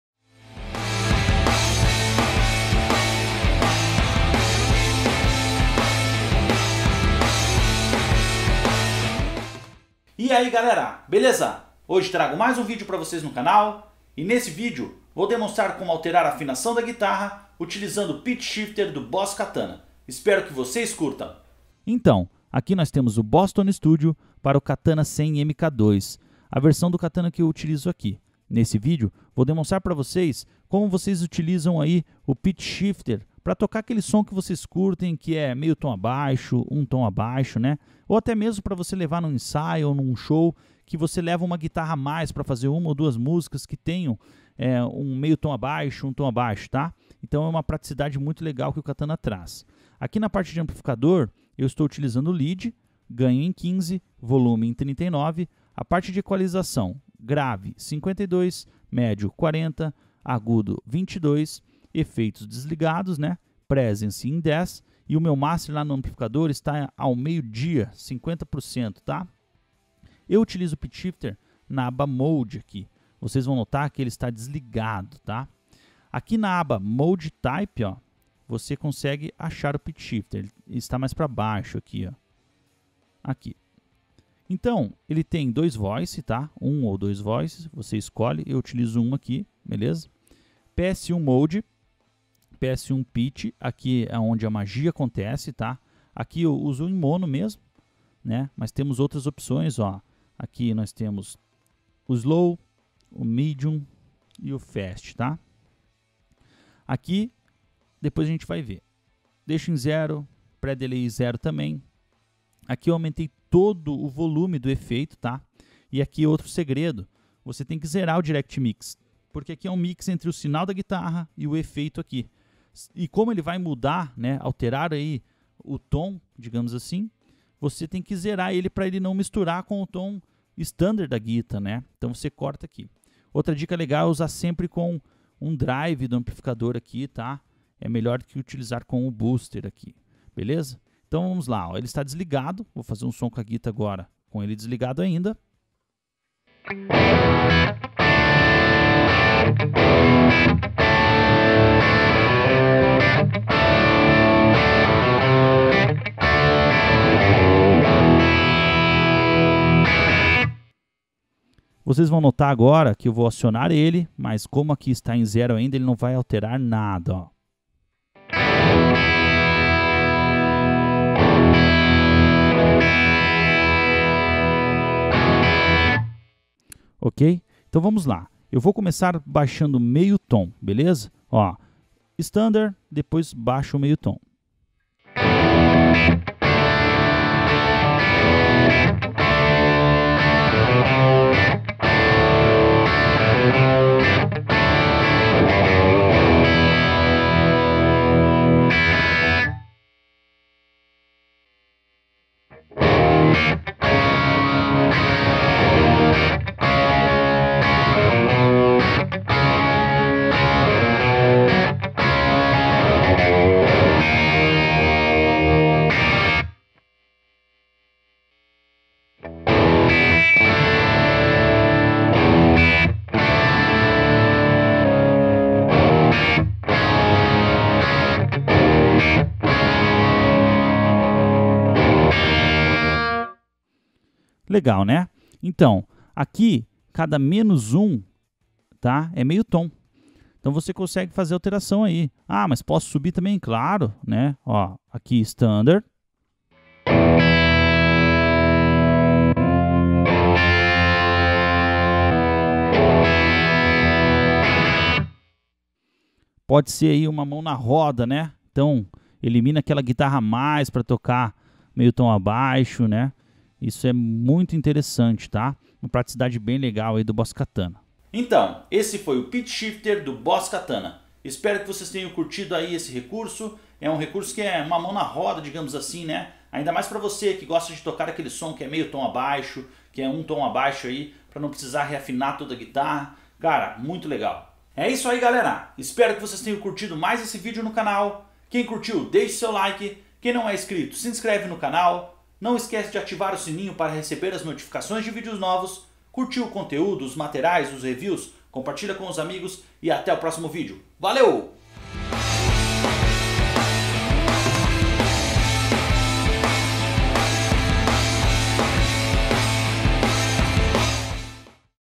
Pitch Shifter Boss Katana MKII - Rodrigo Mellegari
Nesse vídeo demonstro como alterar a afinação da guitarra através do Pitch Shifter do Boss Katana MKII utilizando o Boss Tone Studio. Espero que vocês curtam.